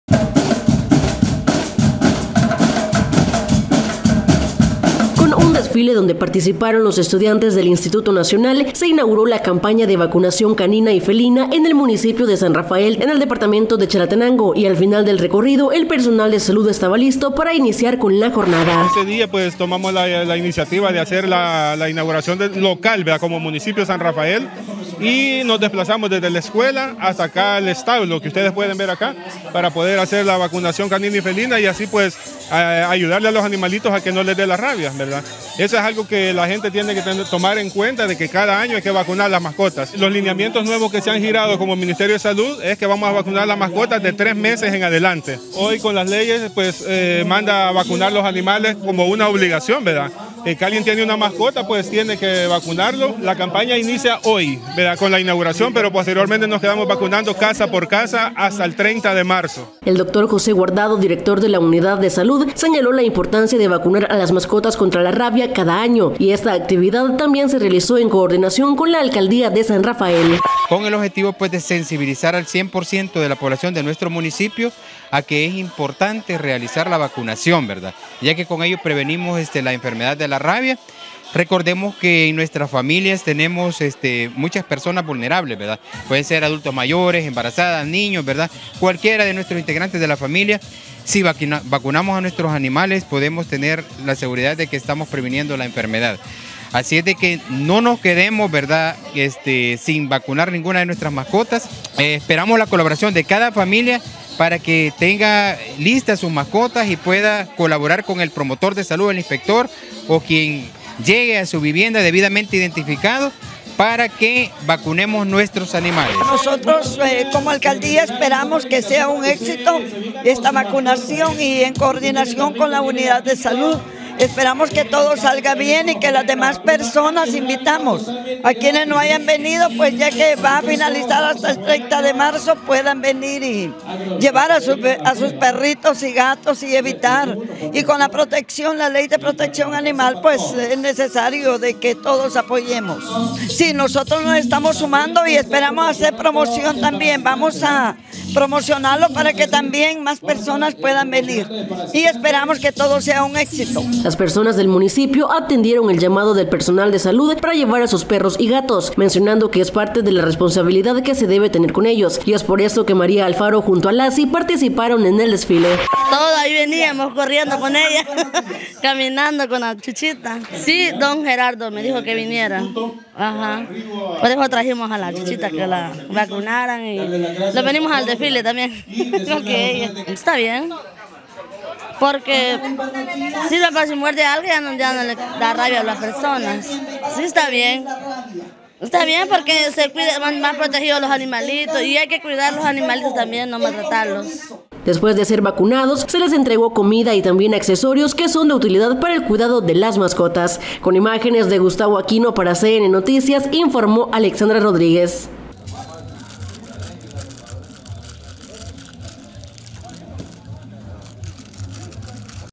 CAMPAÑA DE VACUNACION ANTI RABICA PARA CANINOS Y FELINOS EN SAN RAFAEL
En el municipio de San Rafael en Chalatenango se inauguró oficialmente la campaña de vacunación contra la rabia en caninos y felinos, el personal de la Unidad de Salud organizó un desfile con la participación de estudiantes y apoyo de la alcaldía municipal, iniciando así con la jornada.